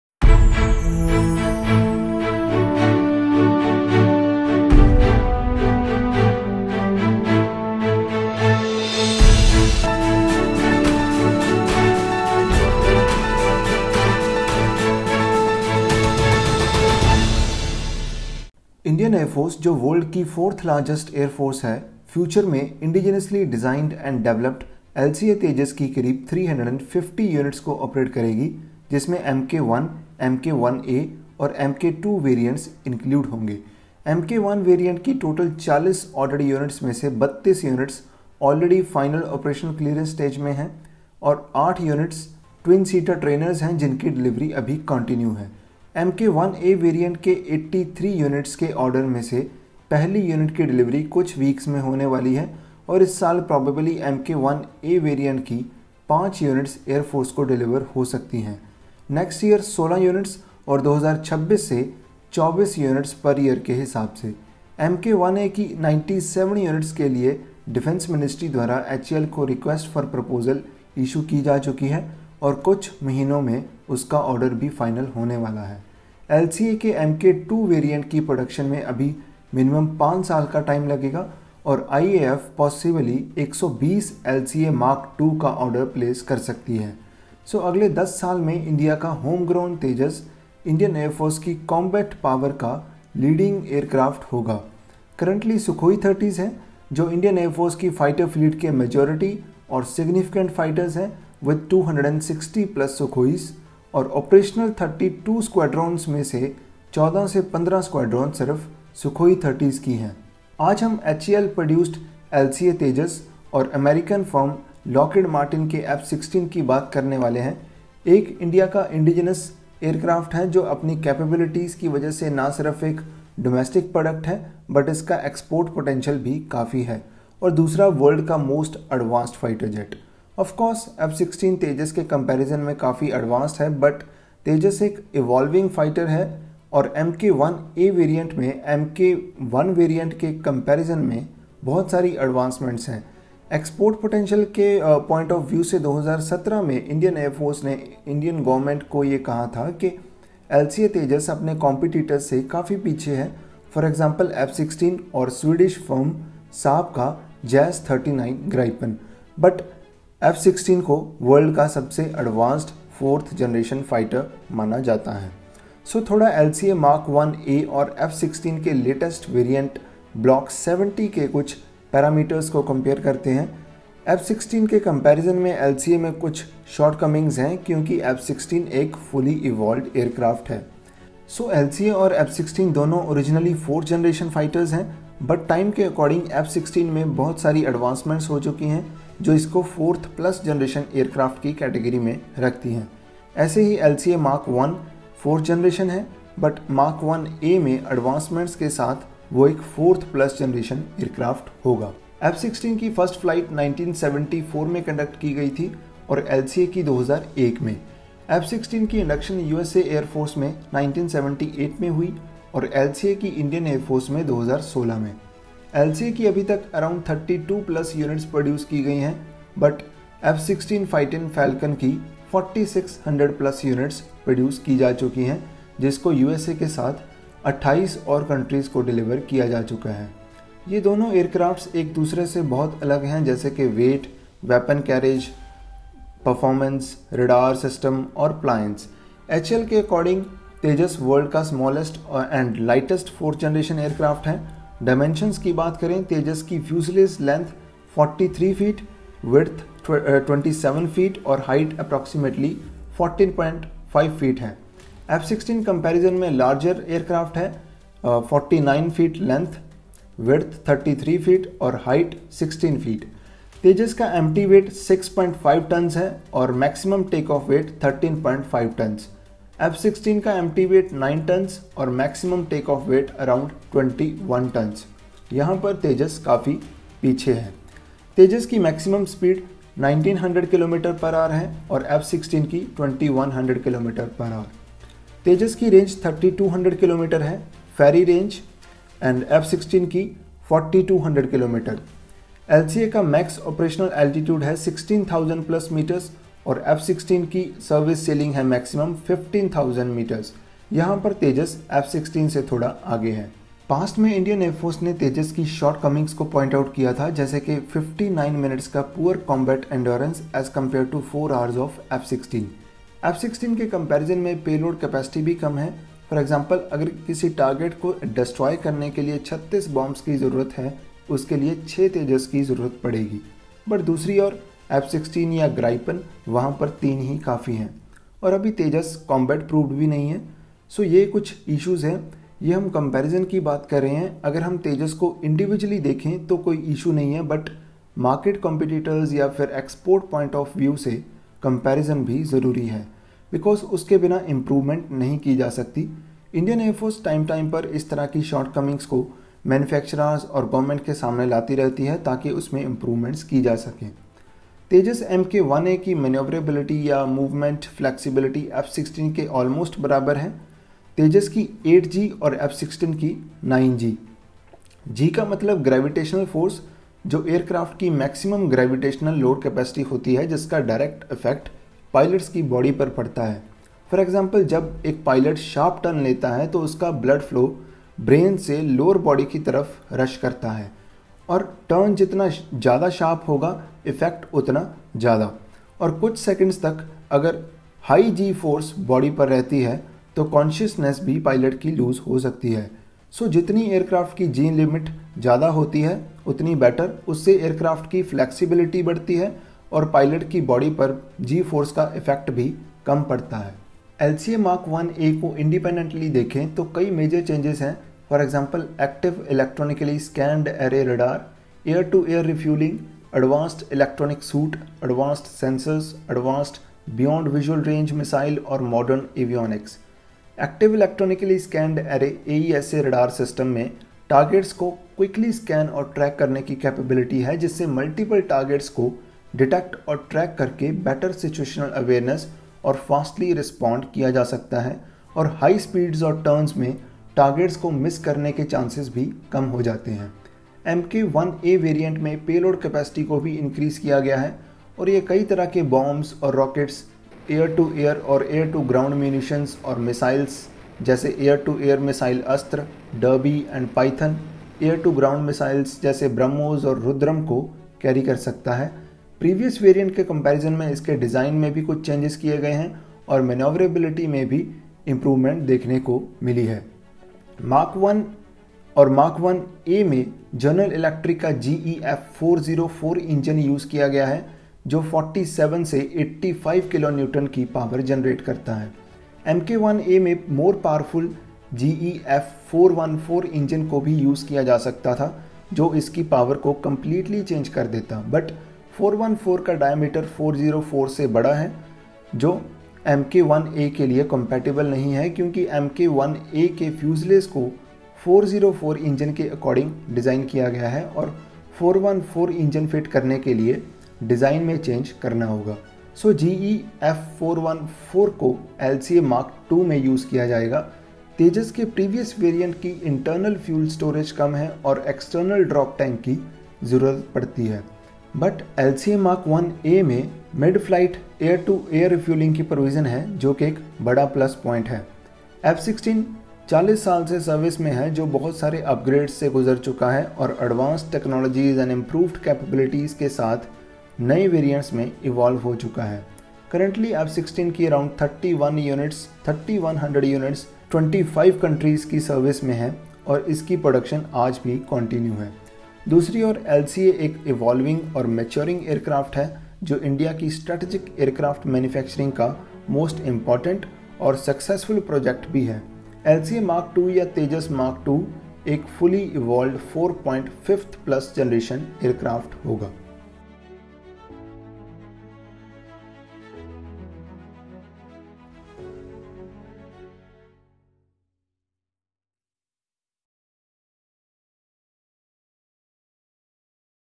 Light combat aircraft - Tejas, which is undergoing through advancements and the F-16 fighting falcon
The Tejas, a light combat aircraft, is poised to become the primary fighter jet for the Indian Air Force in the upcoming decade. Over the next few years, the Indian Air Force plans to integrate a total of 350 light combat aircraft, including variants such as MK1, MK1A, and MK2, into its fighter fleet.

Despite being inducted into the Indian Air Force just a decade ago, the Tejas is continually evolving, undergoing various advancements.

In this video, we have discussed the Tejas alongside the world's most advanced fourth-generation fighter aircraft, the F-16 Fighting Falcon.

 ✅ Credits to (for the images/Clips used in the video for representational purposes):

       ➡️ Indian Air Force
       ➡️ HAL